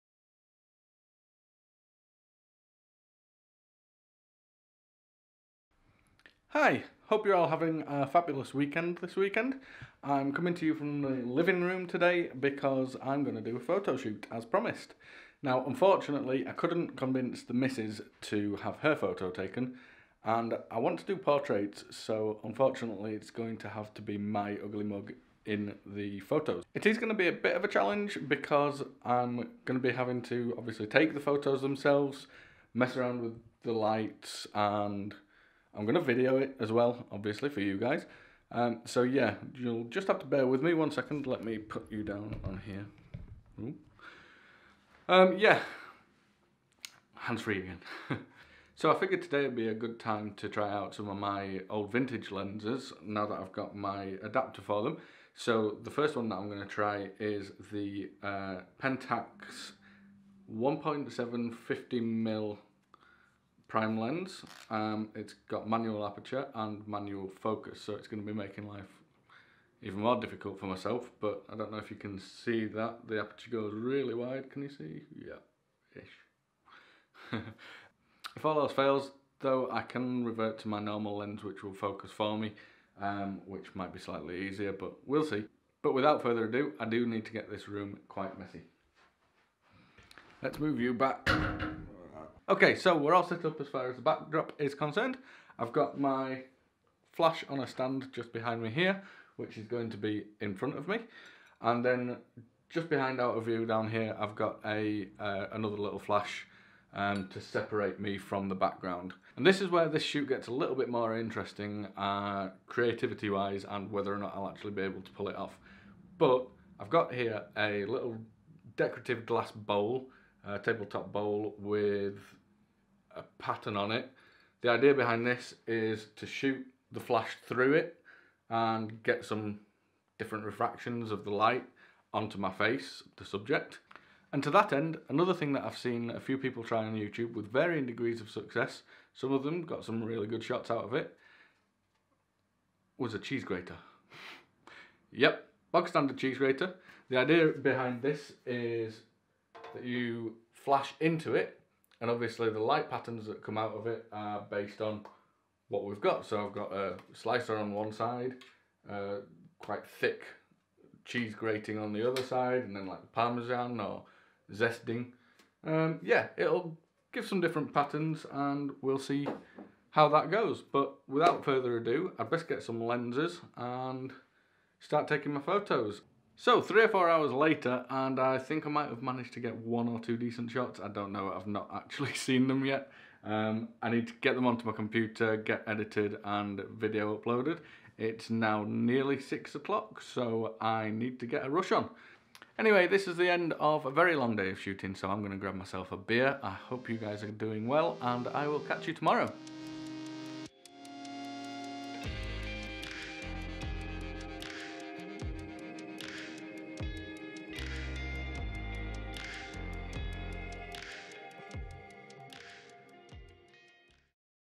UK Lockdown Day 33 (Photography with a cheesegrater!)
This was probably one of the longest most stressful shoots I've done so far - turns out I suck as a model!!